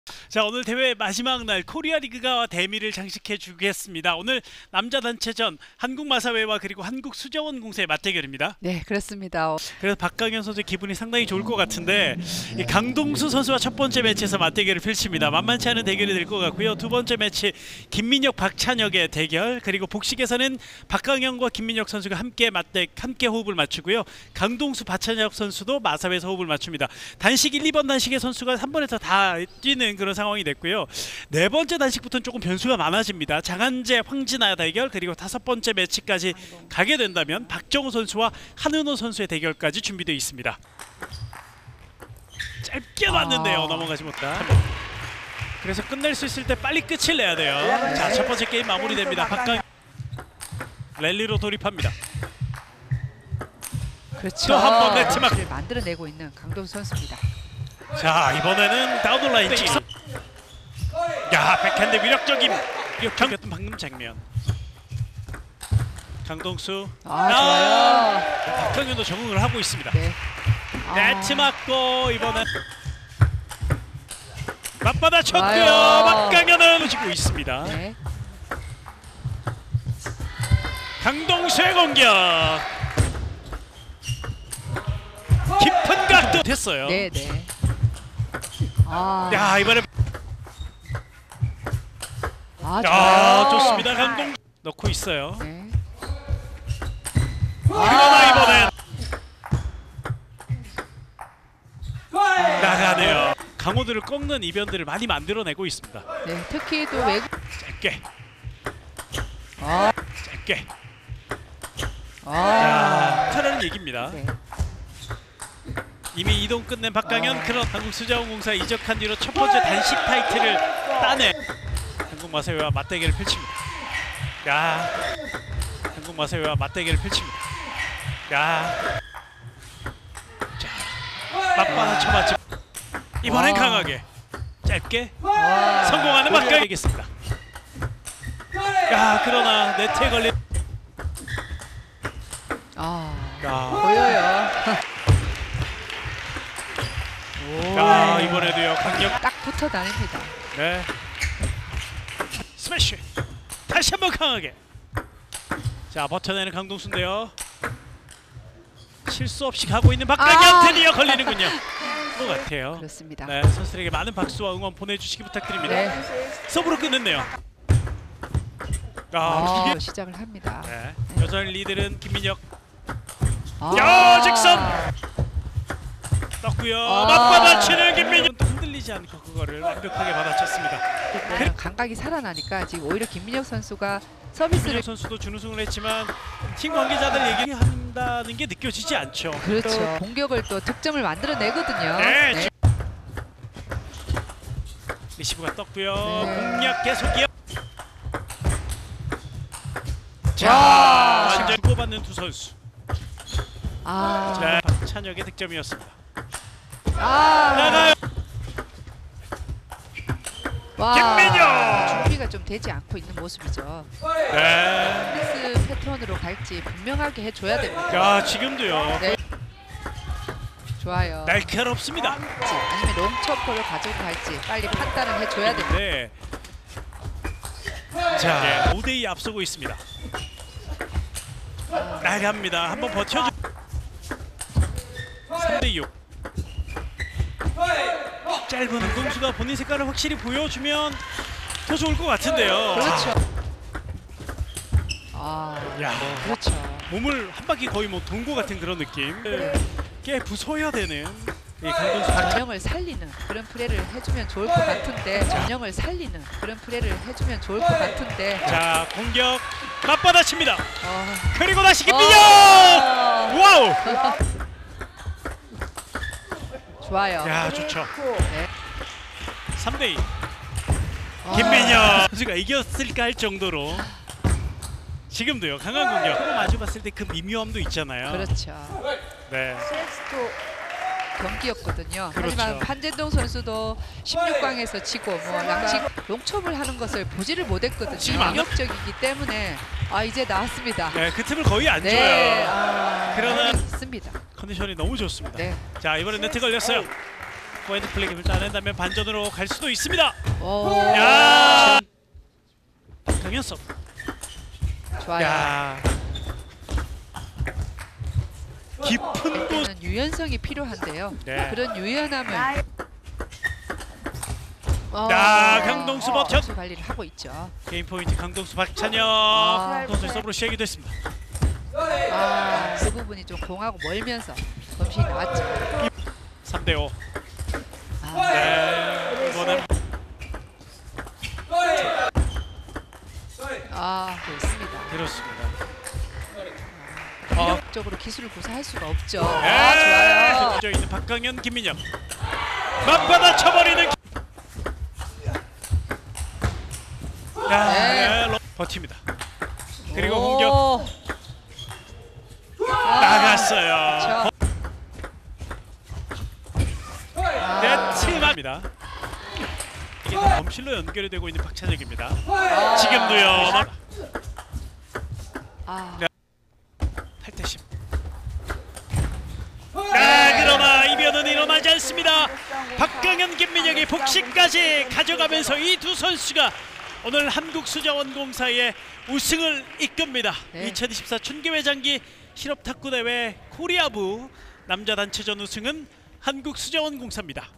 [H/L] 2024 춘계 회장기 실업탁구대회 코리아 남자 단체전 결승 한국수자원공사 vs 한국마사회 - 2024.05.11
2024 춘계 회장기 실업탁구대회

일정 : 2024년 5월 6일(월) ~ 5월 10일(토) 6일간
장소 : 제천시어울림체육센터